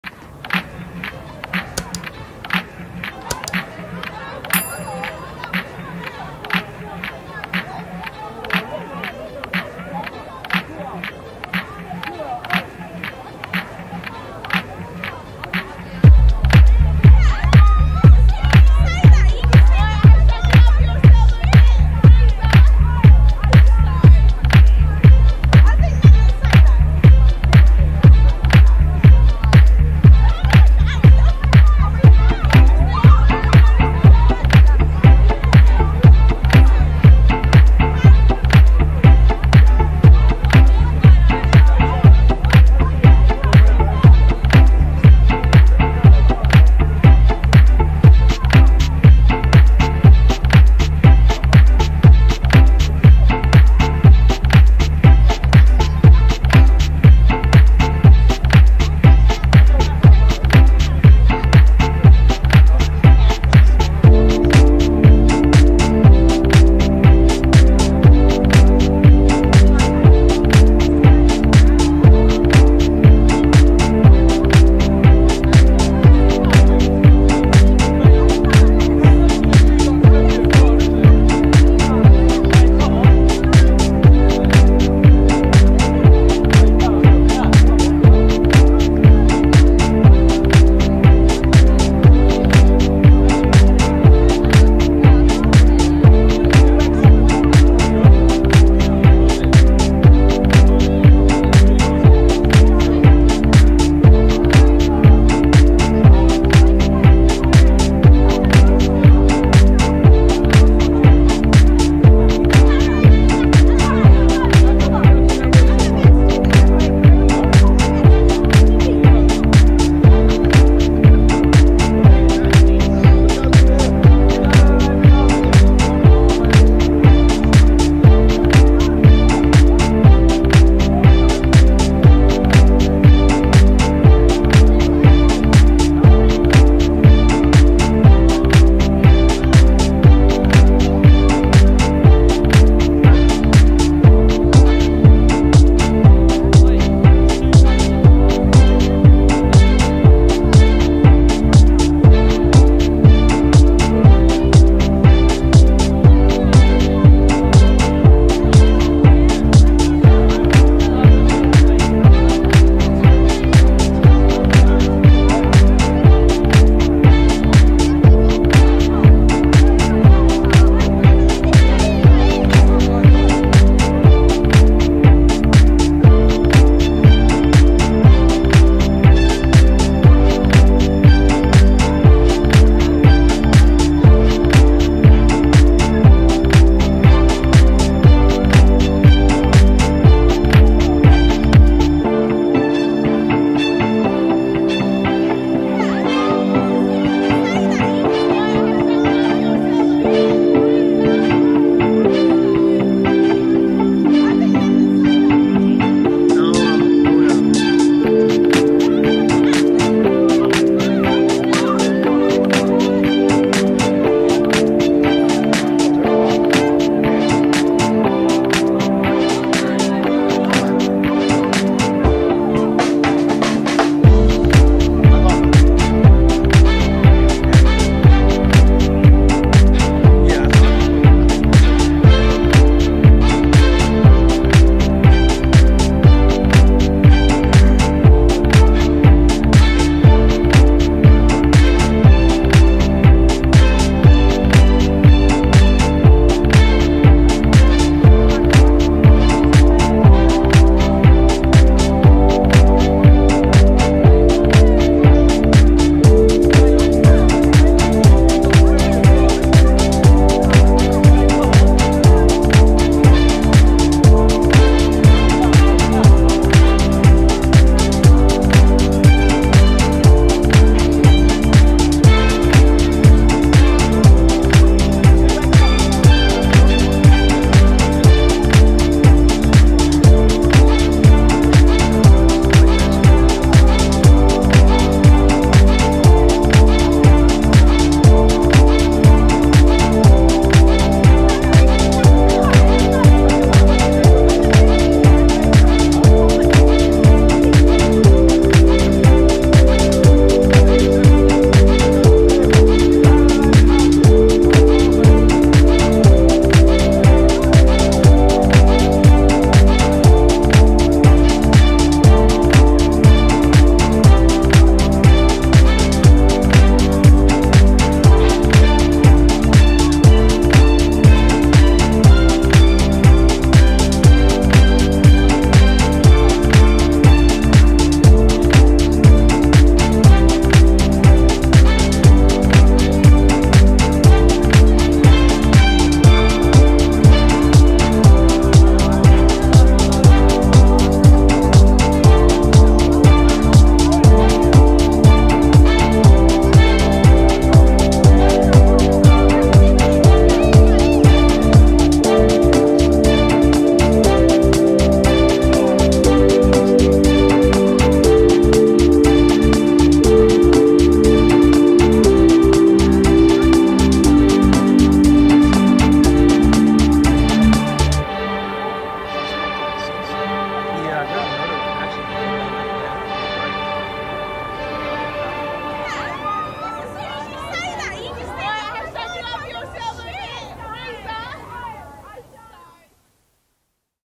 The Roanoke, Virginia Skyline At Sunset 🌆! Relaxing City Music!
Standing on top of Mill Mountain and looking out at the vast city of Roanoke while the sunsets! It is Springtime and city life has certainly kicked in! Also sit back and relax to this city urban music!